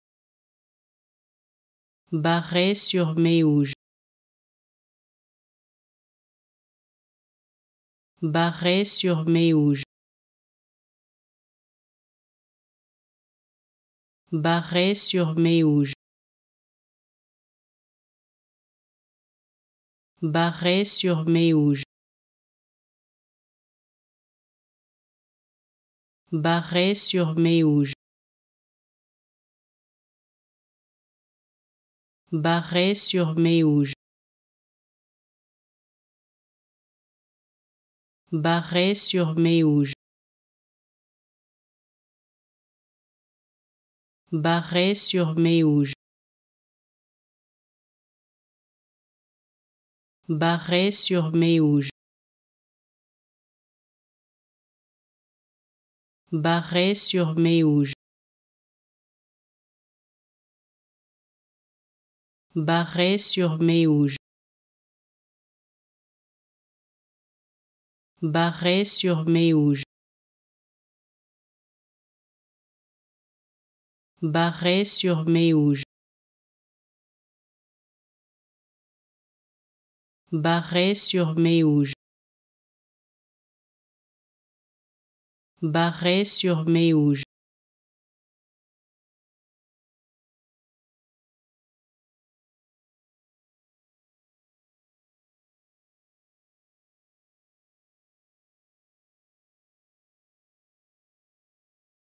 How to Pronounce ''Barret-sur-Méouge'' Correctly in French
Learn how to say and properly pronounce ''Barret-sur-Méouge'' in French with this free pronunciation tutorial.